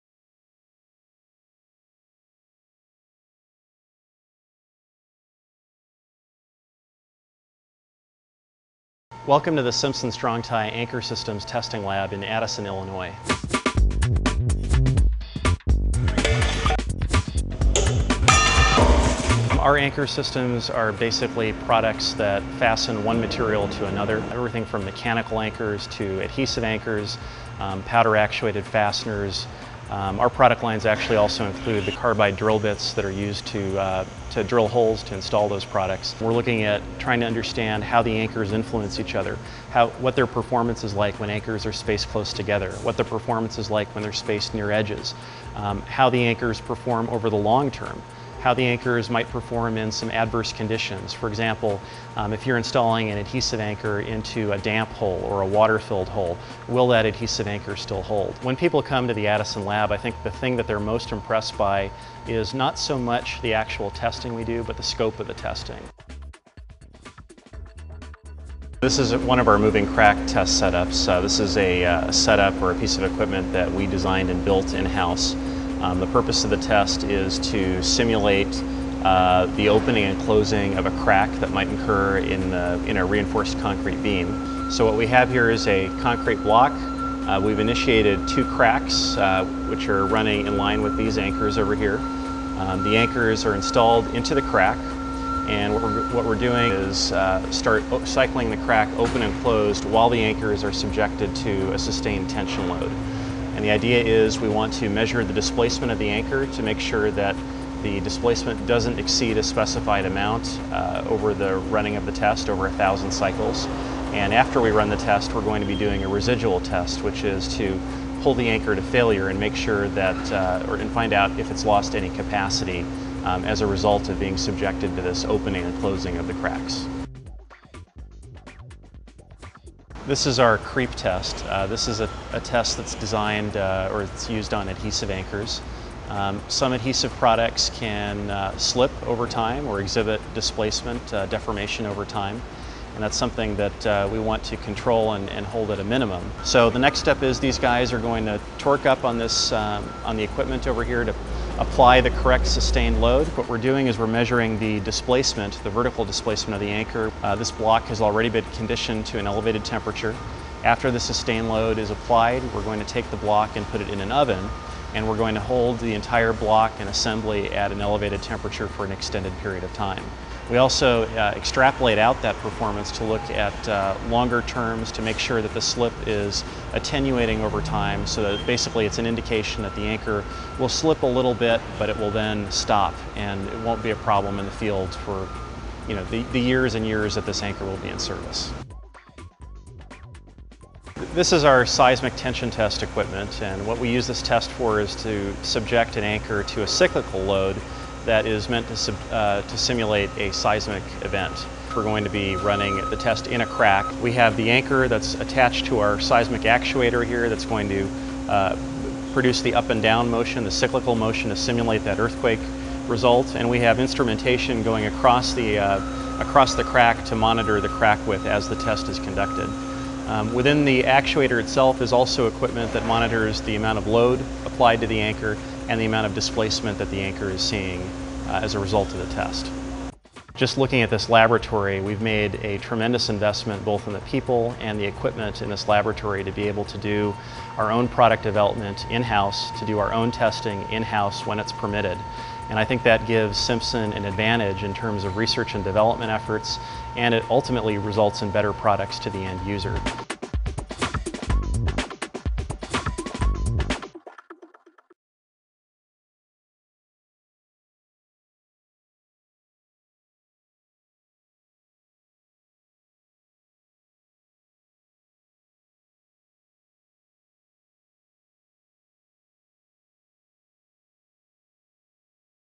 Anchor Systems Lab
The Anchor Systems Lab tests the structural integrity of concrete structures. We use our onsite equipment to simulate earthquakes and other forces that can cause concrete to crack. to learn more about our Anchor Systems. For more information about our research labs,